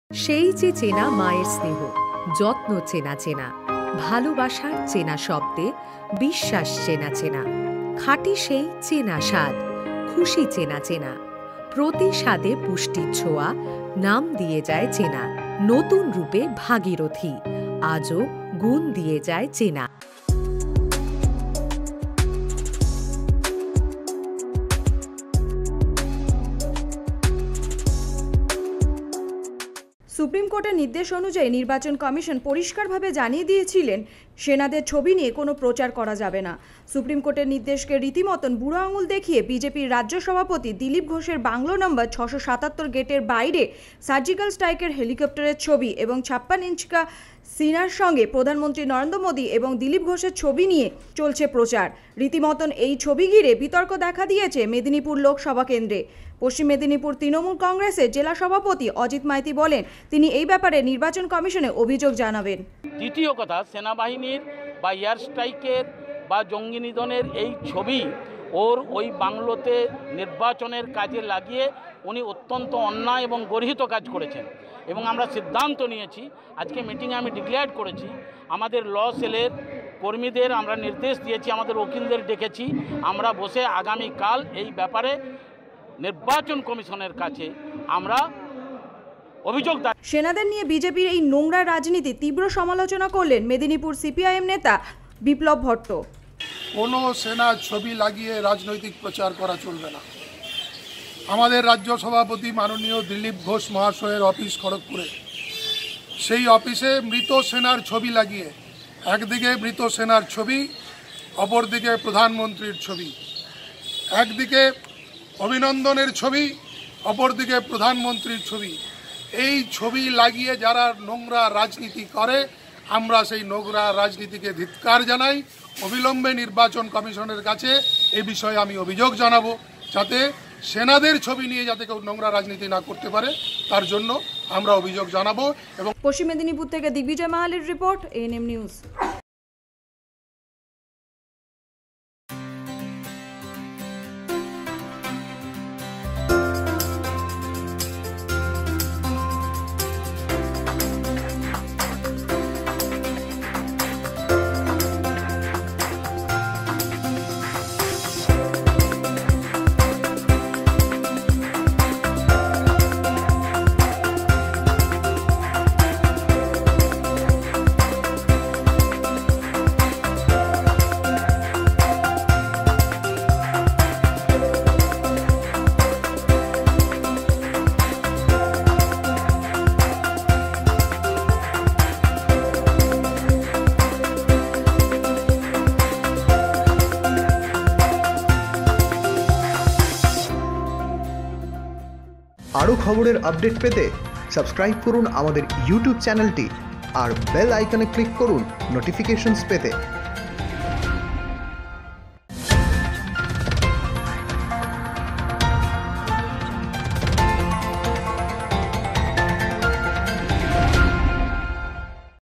দিলীপ ঘোষের বাংলোর সামনে সেনার ছবি নিয়ে তীব্র বিতর্ক ||17032019||ANM NEWS
দিগ্বিজয় মাহালী ,পশ্চিম মেদিনীপুর :সুপ্রিম কোর্টের নির্দেশ অনুযায়ী নির্বাচন কমিশন পরিষ্কারভাবে জানিয়ে দিয়েছিল সেনা দের ছবি দিয়ে কোন প্রচার করা যাবে না।সুপ্রিম কোর্টের নির্দেশেকে রীতিমতন বুড়ো আঙুল দেখিয়ে বিজেপির রাজ্য সভাপতি দিলীপ ঘোষের বাংলো নাম্বার ৬৭৭ গেটের বাইরে সার্জিক্যাল স্ট্রাইক এর হেলিকপ্টারের ছবি,ছাপ্পান ইঞ্চি কা সিনা সঙ্গে প্রধানমন্ত্রী নরেন্দ্র মোদী এবং দিলীপ ঘোষের ছবি দিয়ে চলছে প্রচার ।রীতিমতন এই ছবি ঘিরে বিতর্ক দেখা দিয়েছে মেদিনীপুর লোকসভা কেন্দ্রে।পশ্চিম মেদিনীপুরের তৃণমূল কংগ্রেসের  জেলা সভাপতি অজিত মাইতি বলেন তিনি এই ব্যাপারে নির্বাচন কমিশনে অভিযোগ জানাবেন।